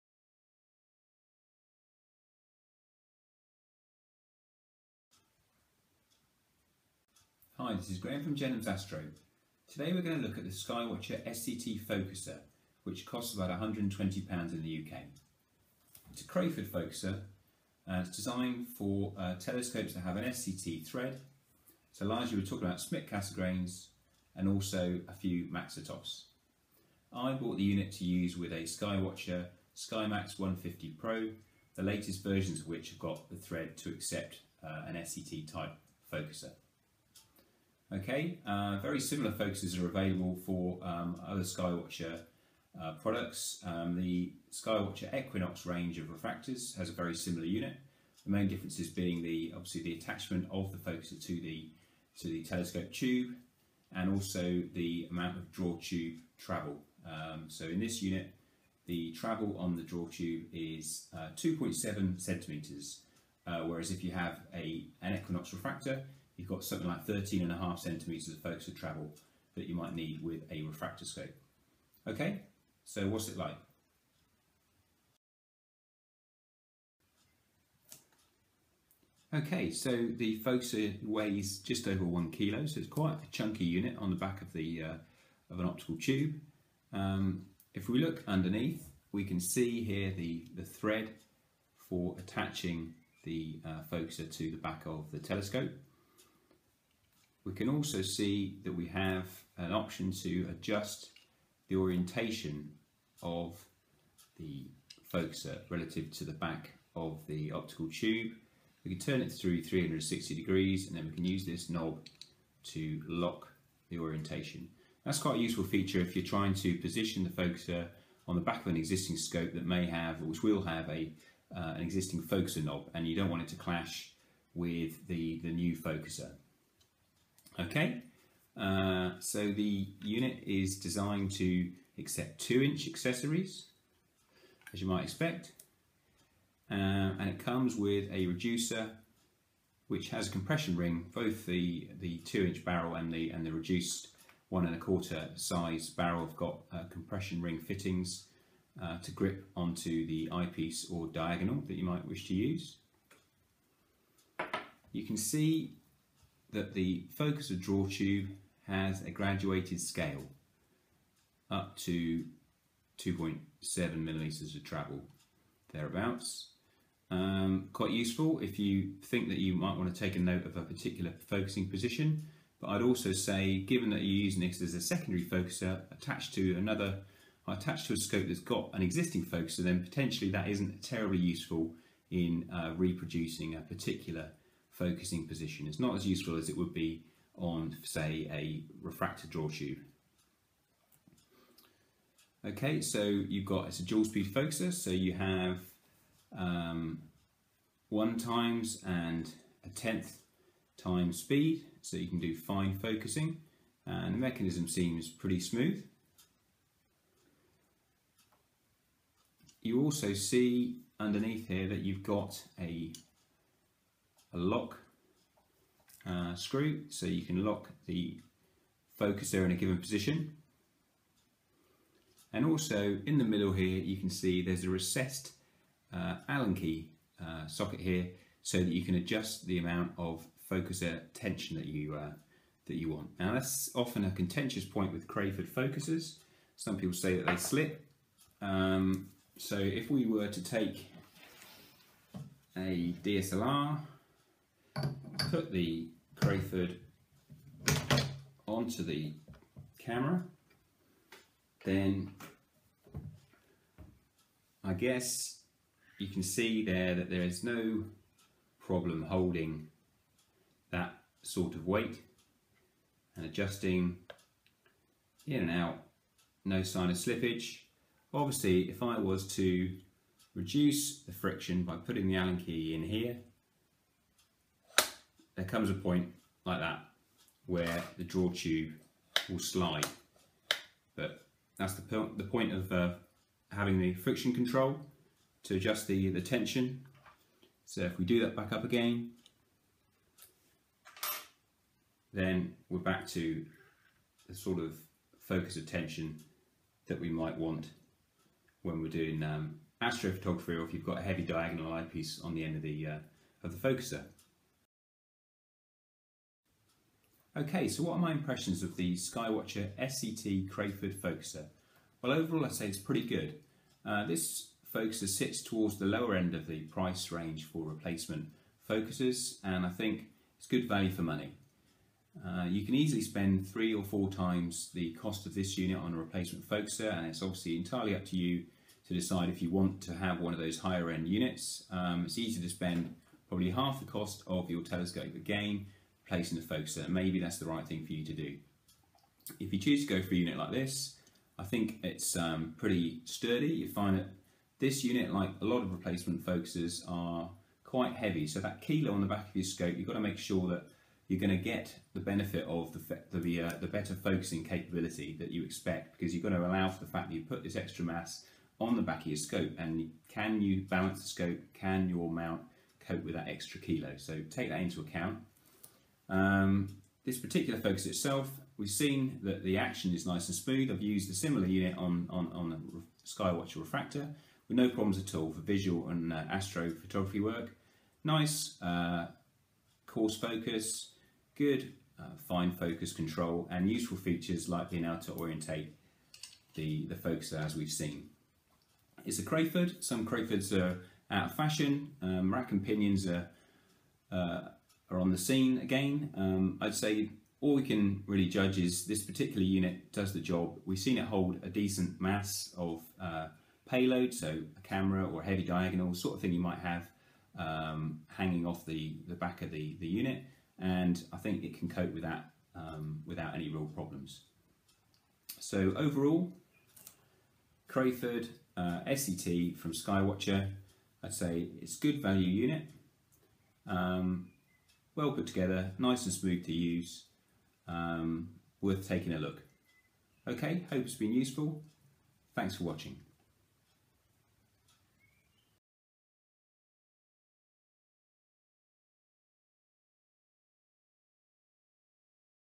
Skywatcher Crayford Focuser for SCT
A review of the Skywatcher Crayford replacement focuser, for telescopes which have an SCT thread.